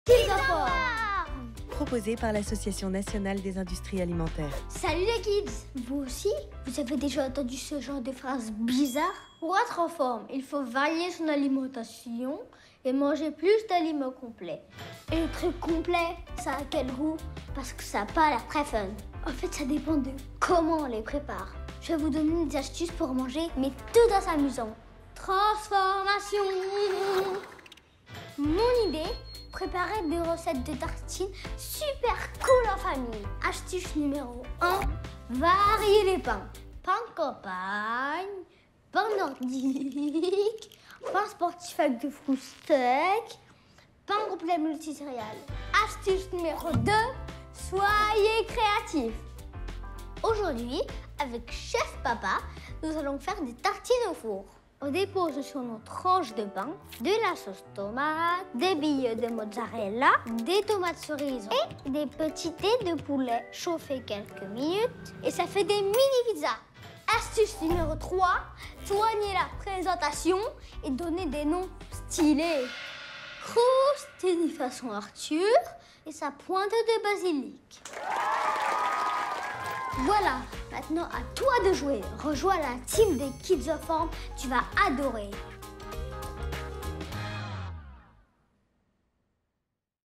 Kid'z en forme - Episode 4 : La consommation de légumes et de céréales complètes
Pour les enfants, les aliments complets sont une super source d’énergie dont le corps a besoin, et ils se déclinent en multiples saveurs pour varier les plaisirs. Alors comment les intégrer facilement au quotidien ?
Chef Arthur a la solution ! Il invite les enfants à cuisiner en famille et nous livre une de ses recettes simple et gourmande à base de pain complet !

À l'occasion de la journée de lutte contre l'obésité, regarde toutes les vidéos Kid'z en forme pour avoir de bons conseils pour une alimentation variée et équilibrée.